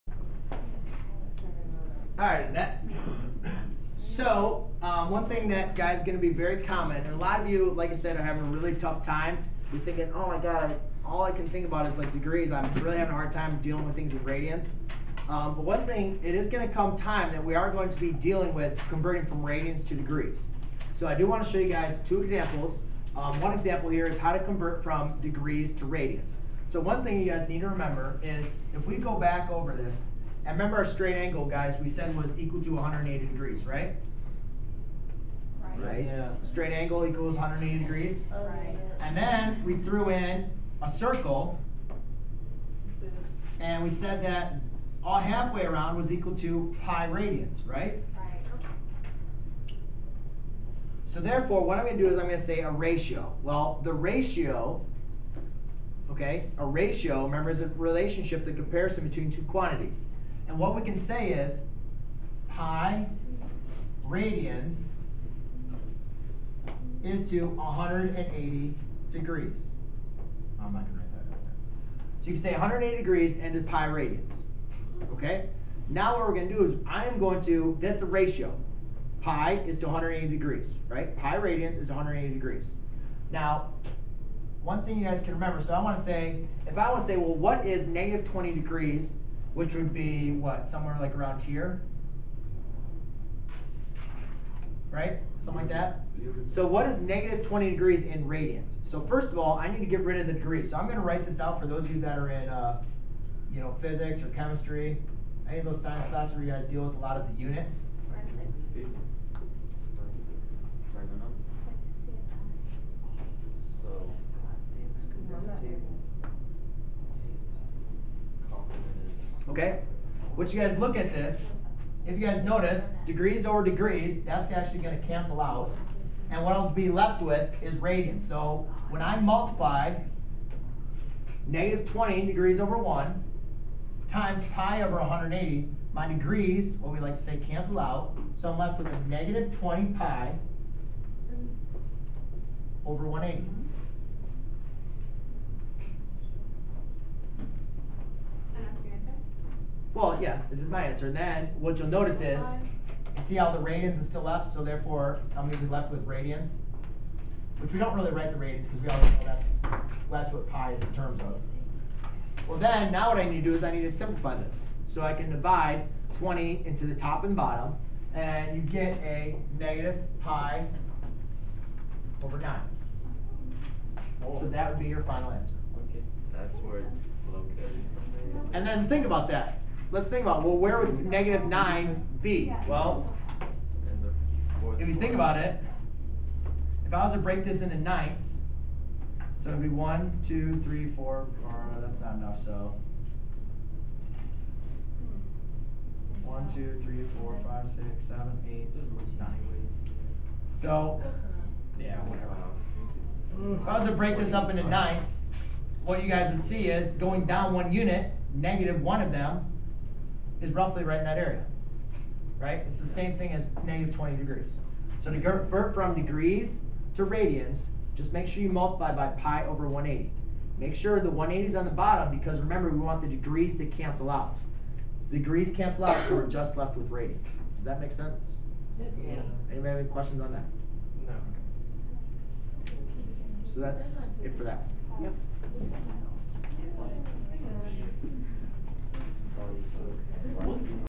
Converting a negative angle in degrees to radians
👉 Learn how to determine co-terminal angles given one angle. An angle is a figure formed by two rays that have a common endpoint. The two rays are called the sides of the angle while the common endpoint is called the vertex of the angle. We measure angles starting from the positive x-axis, i.e. the initial side of an angle measure is usually the positive x-axis. The angle measured in the anti-clockwise direction is called a positive angle while a negative angle is measured in the clockwise direction.

Coterminal angles are angles that have the same initial and terminal sides. Coterminal angles are found by adding/subtracting 360 degrees (for degree angle measure) or 2pi (for radian angle measure) to/from the given angle.  Usually, we are looking for the smallest positive and smallest negative coterminal angles since there are infinitely many answers.